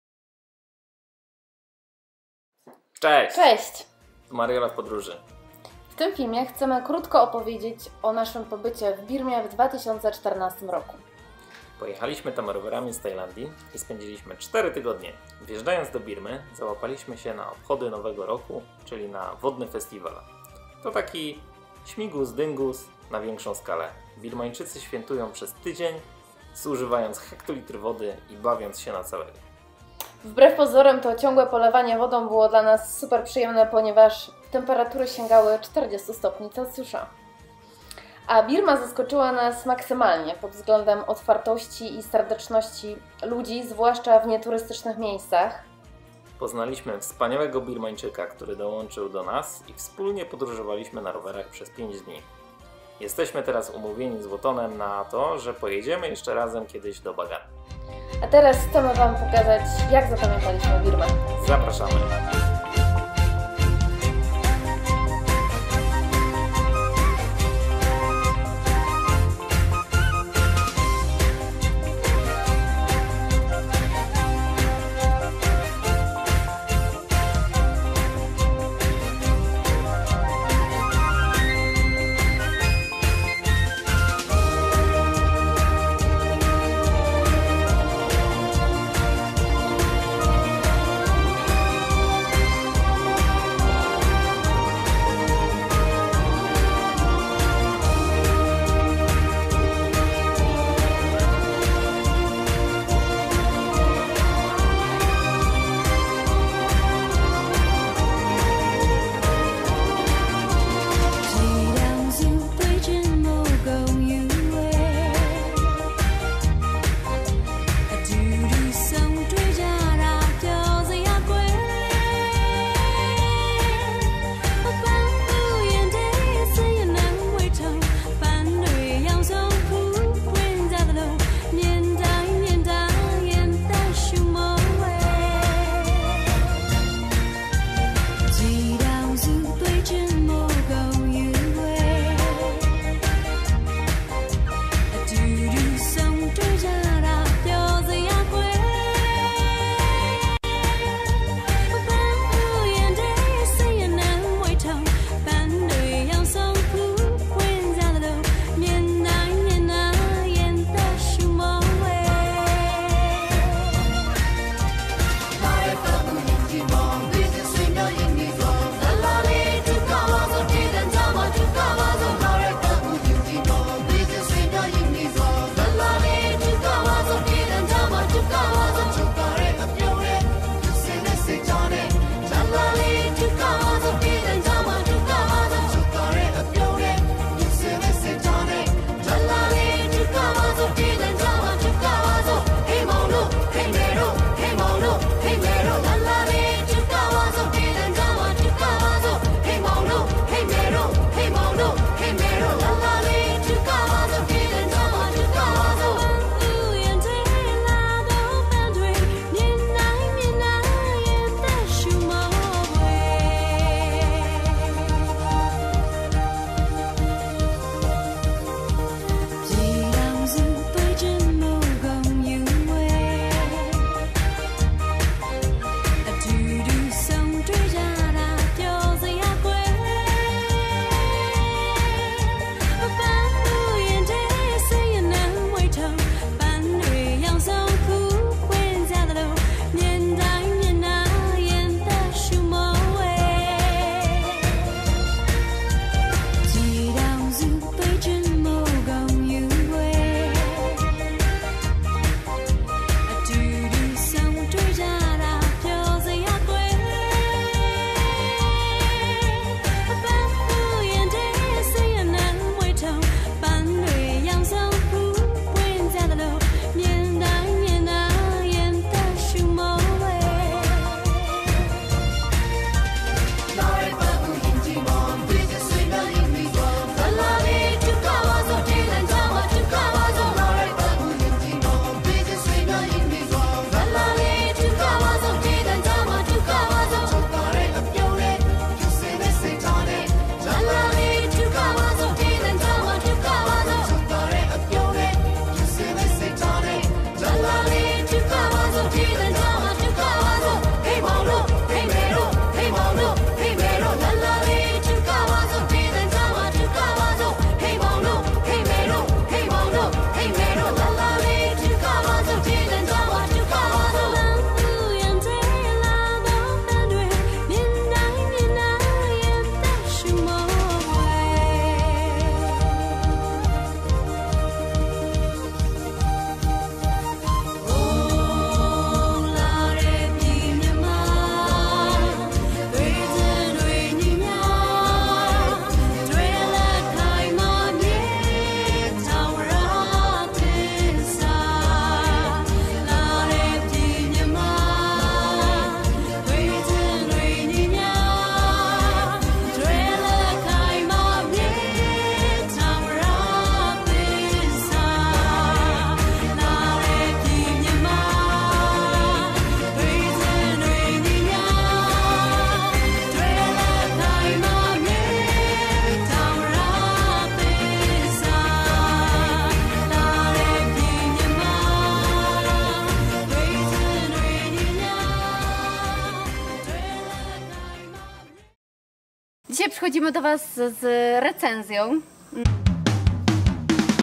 MariolaWpodrozy Birma wspomnienia 2014
Cześć,
Chcesz zobaczyć jak podróżuje się po Birmie? Koniecznie obejrzyj ten film:)
Pozdrawiamy,
MariOla